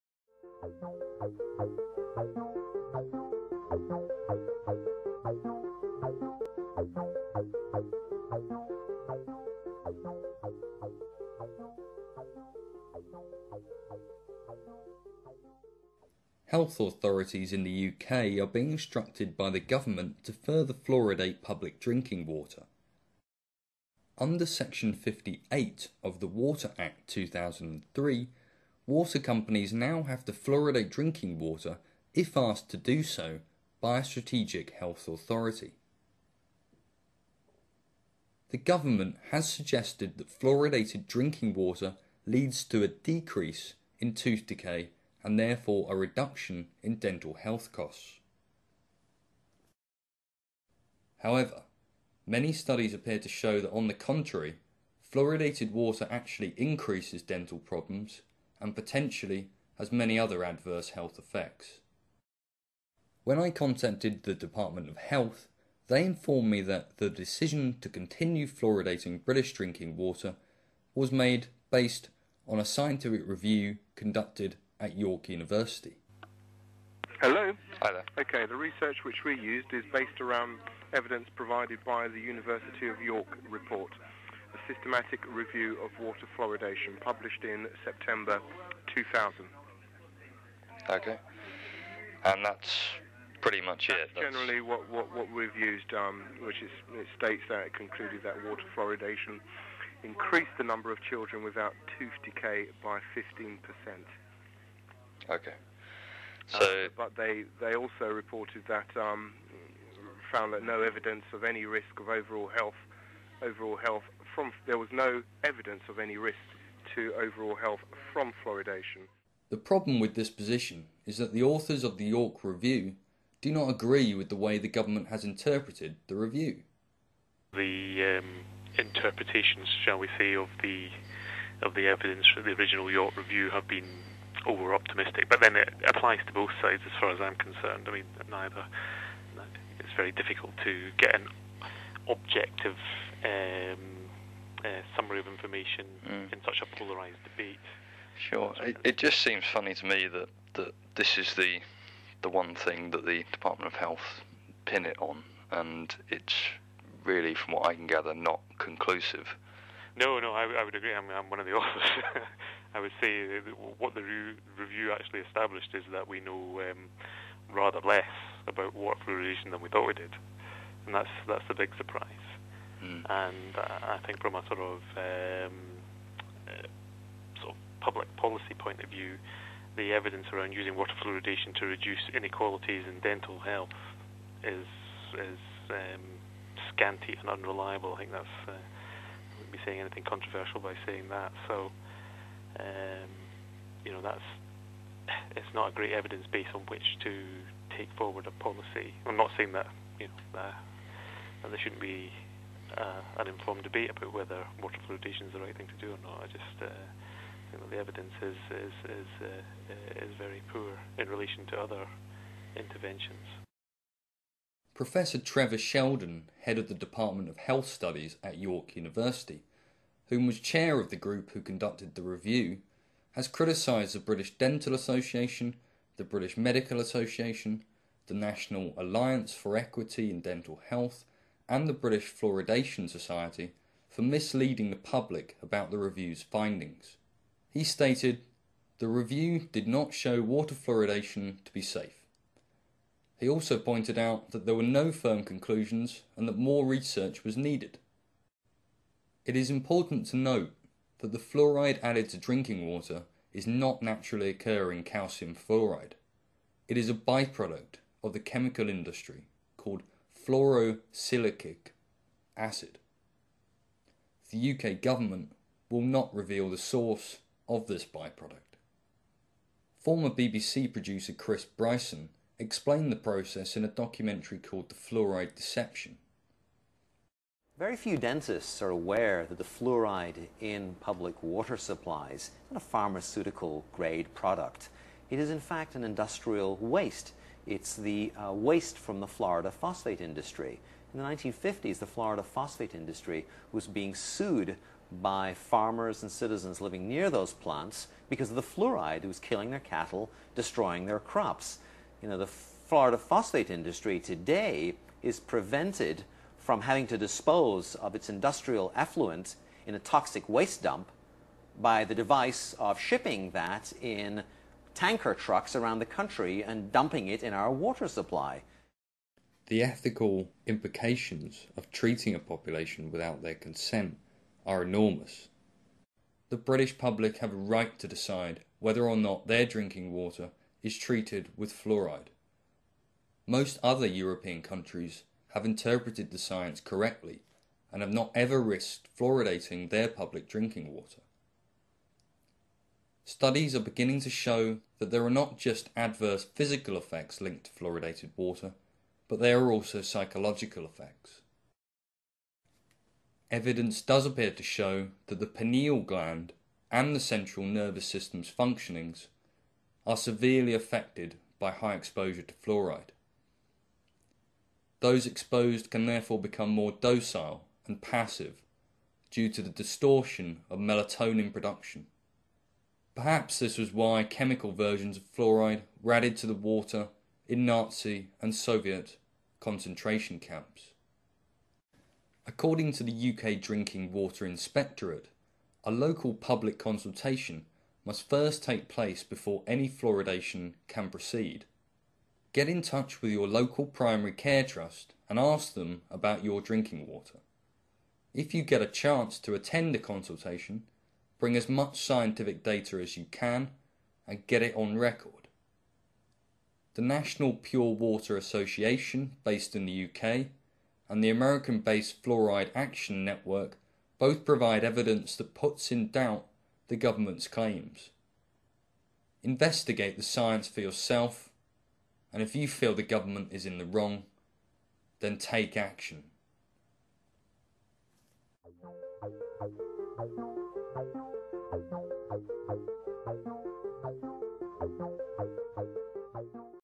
Czy fluor w wodzie pitnej w Wielkiej Brytanii jest bezpieczny? [napisy PL]
Fluor dodawany do wody nie jest naturalnie występującym fluorkiem wapnia, jest to produkt uboczny przemysłu chemicznego,
zwany kwasem heksafluorokrzemowym.
To odpad przemysłowy  z fabryk zajmujących się produkcją fosforanów.

I have done subtitles for this video.
I do not own nor claim any rights to this content. I am publishing this solely for informational and educational purposes.